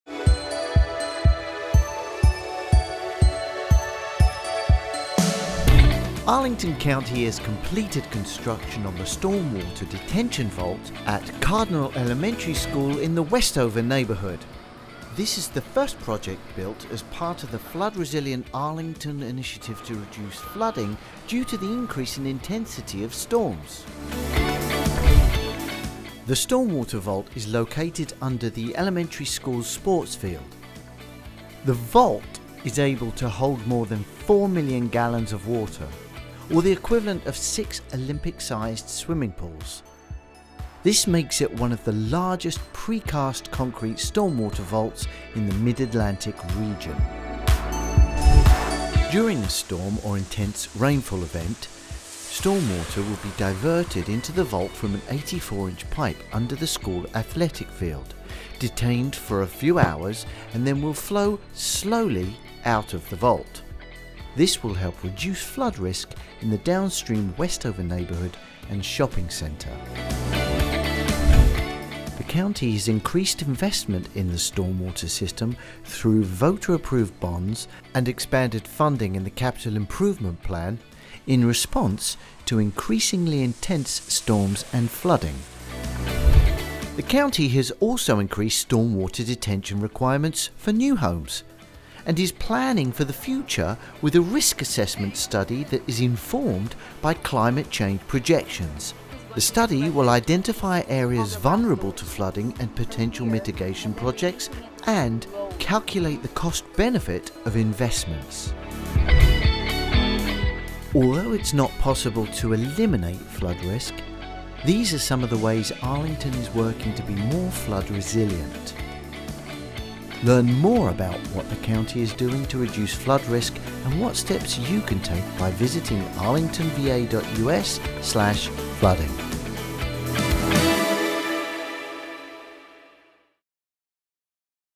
Projects To Watch | Cardinal Elementary School Stormwater Vault
Sitting under restored playing fields, the 47,000 square-foot vault is capable of temporarily storing over 4 million gallons of water (equal to six Olympic swimming pools), offering a unique Arlington solution to limited public space. Learn how one of the largest precast
concrete stormwater vaults in the Mid-Atlantic region will help reduce flood risk in the Torreyson Run watershed area of Arlington County, Virginia.